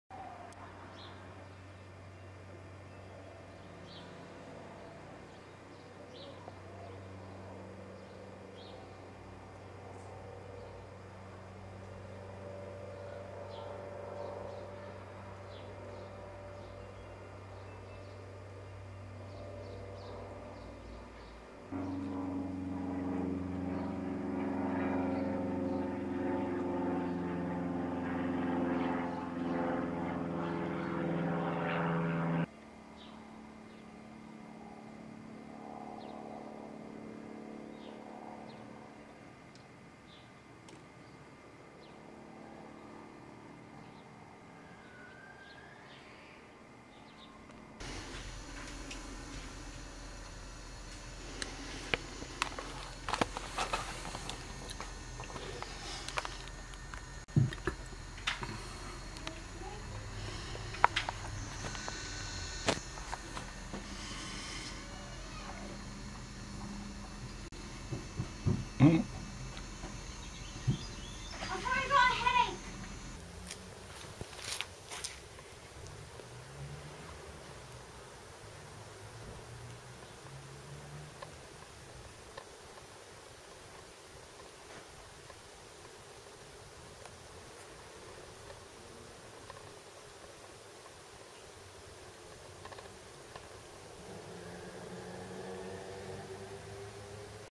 Vlomo 2012 Day 09 - Tree Years
I was poking around in some old footage and found a shot of our back garden in 1996, so I knocked it together with some other, later, shots of our cherry tree.